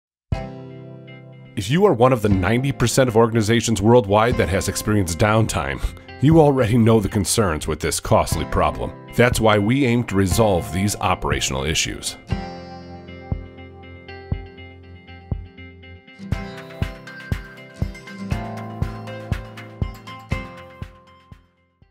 90% of organizations worldwide experience downtime
If you are one of the 90% of organizations worldwide that has experienced downtime, you already know the concerns with this costly problem. That's why we aim to resolve these operational issues.